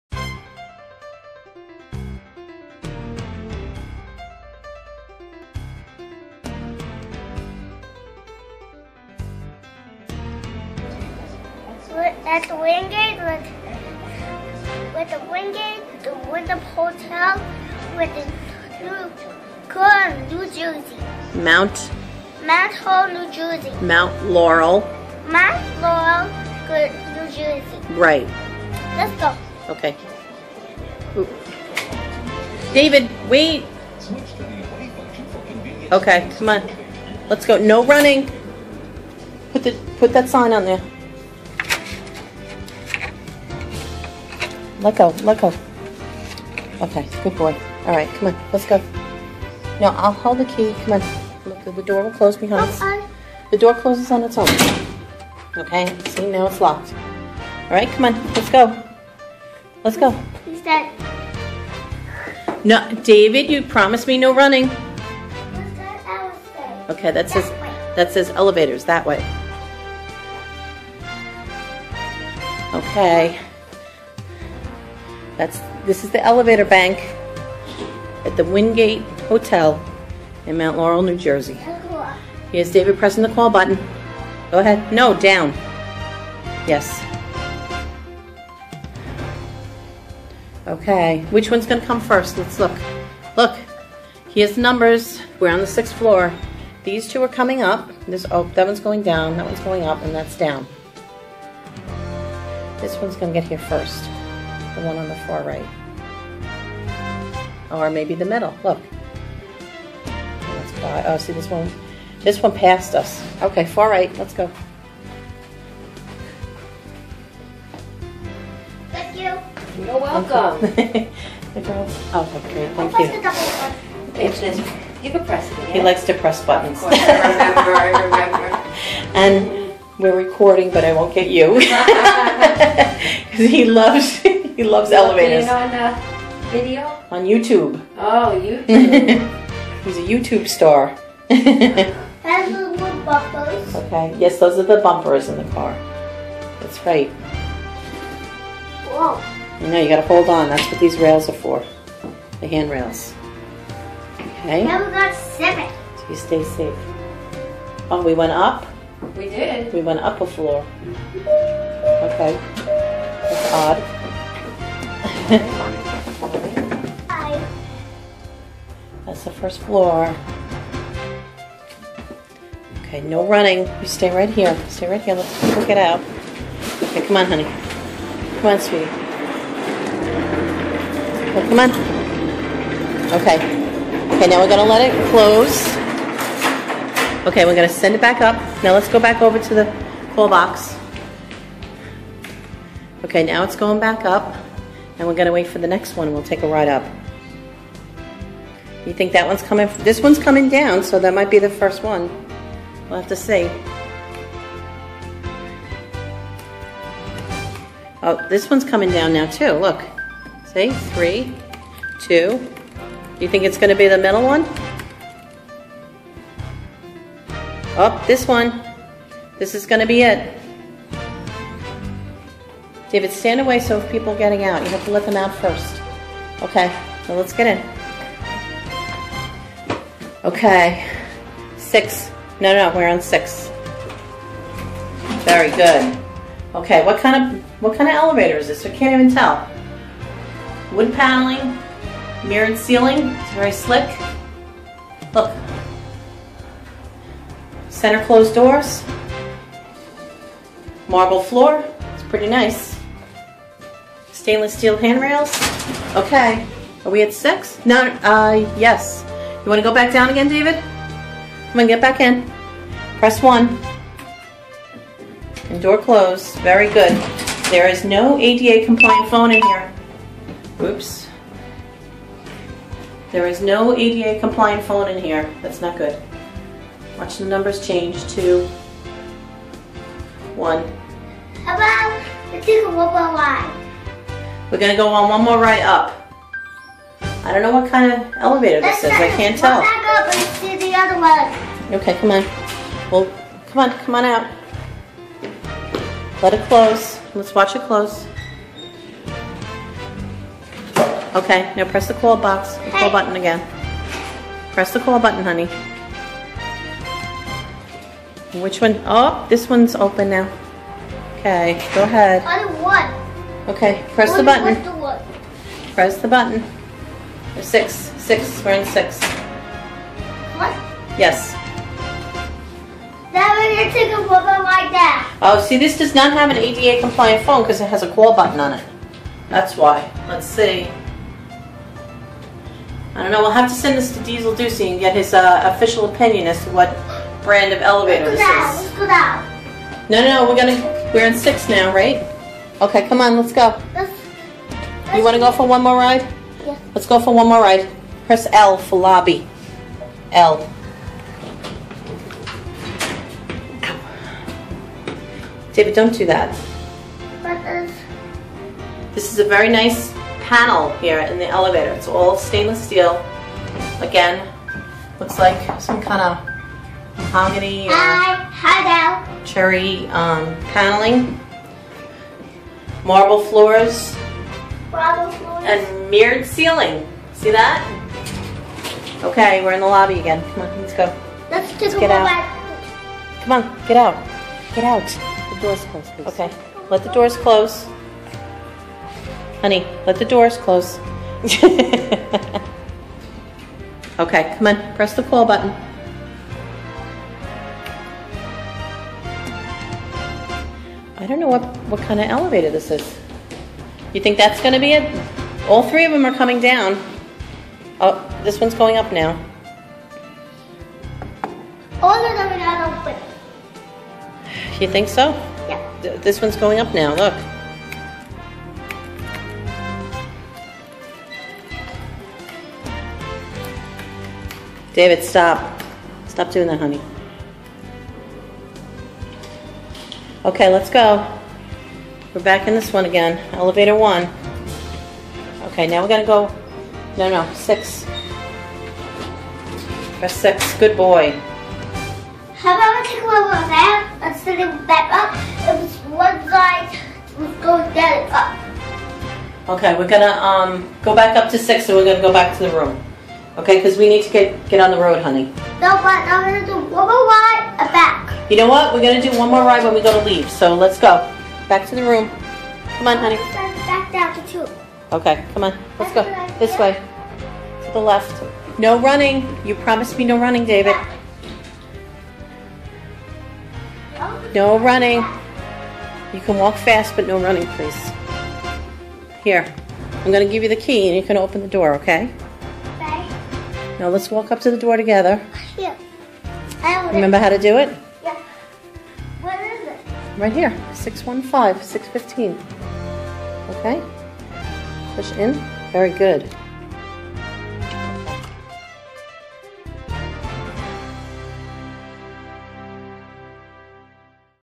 David Reviews a Fine Elevator at the Wingate Hotel New Jersey
David loves elevators and elevator videos so we made another one at the Wingate Hotel in Mount Laurel New Jersey.